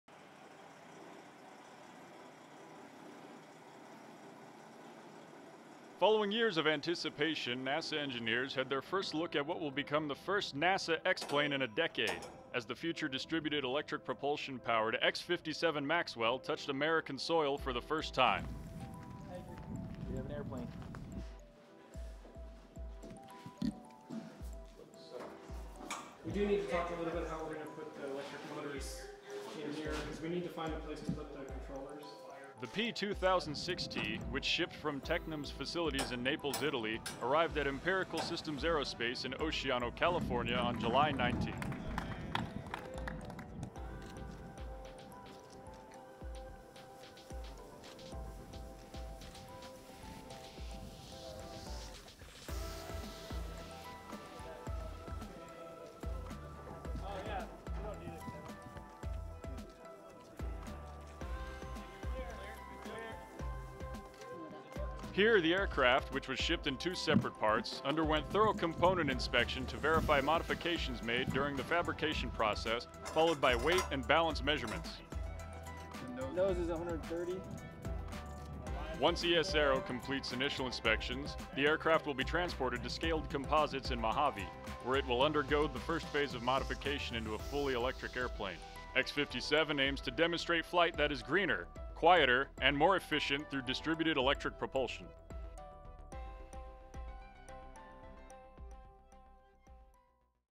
X-57 aircraft arrives in Oceano
This 1-minute, 41-second video shows that NASA’s newest experimental aircraft has arrived in Oceano, CA, and will be transferred to Mojave, CA, to be converted into NASA's first piloted all-electric aircraft. The Italian Tecnam P-2006T fuselage will be mated with a special experimental wing to become the X-57 Maxwell.